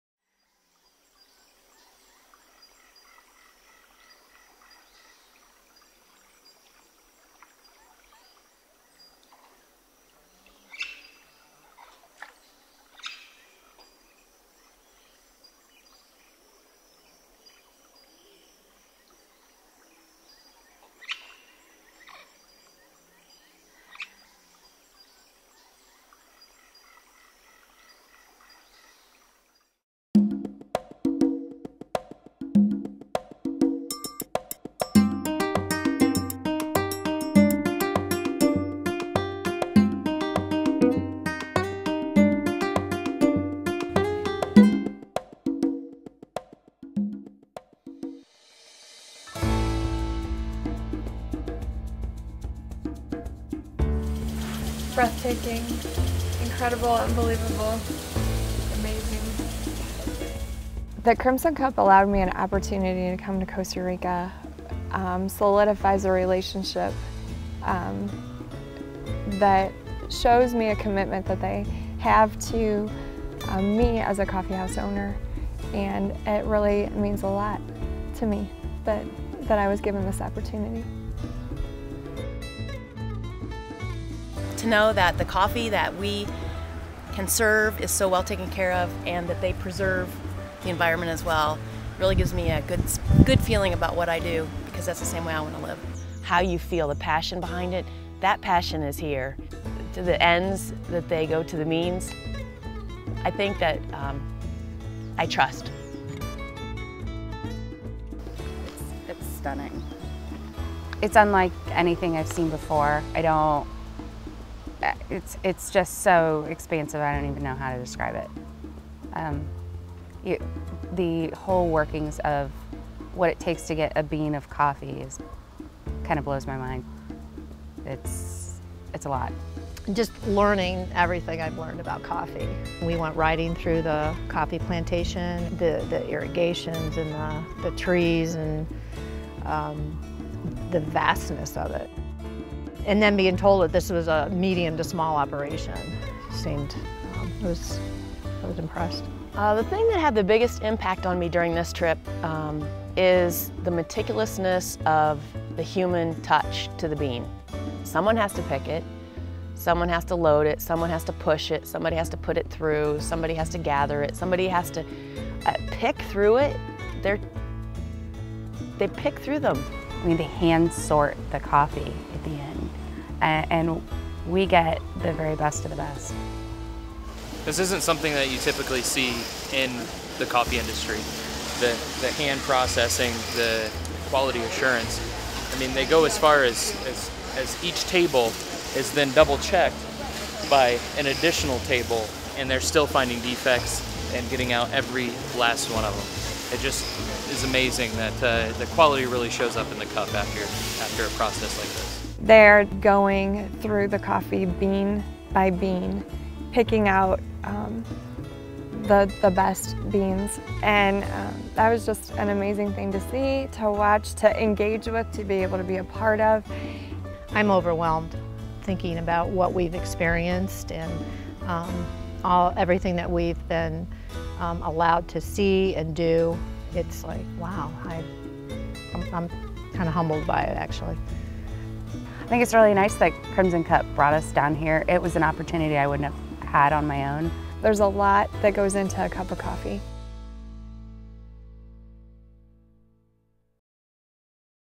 Crimson Cup Coffee - Trip to Costa Rica
Crimson Cup Coffee & Tea, a Columbus, Ohio-based coffee roaster and coffee franchise alternative, recently led a group of four Ohio coffee house owners to the source of some of the world's finest and most sought-after coffees:  Costa Rica's Hacienda Rio Negro and Hacienda La Minita coffee plantations.

Coffee and Training Specialist Brandon Bir, from Crimson Cup led the tour. Participating owners included Tracy Heitmeyer of 5 Bean Coffee (Reynoldsburg), Sheri Thomas of Sheri's Coffee House (Norwalk), Lynne Calvelage of Coffee Amici (Findlay) and Deborah Fulks of Court Street Coffee (Athens).